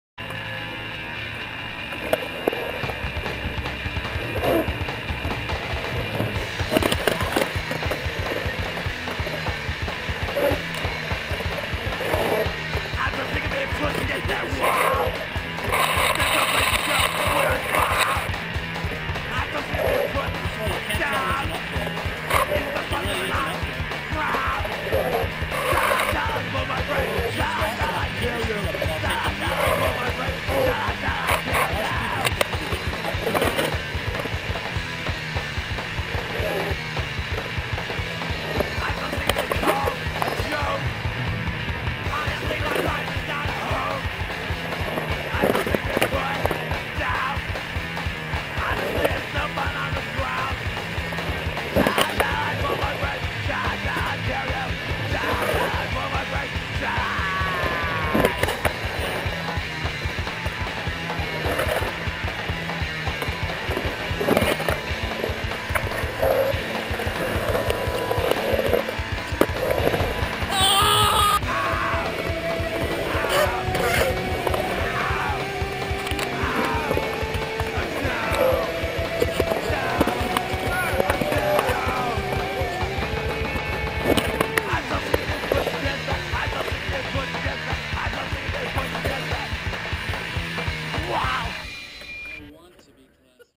Durham Independent Day (second version)
Skateboarding at Durham on July 4, 2012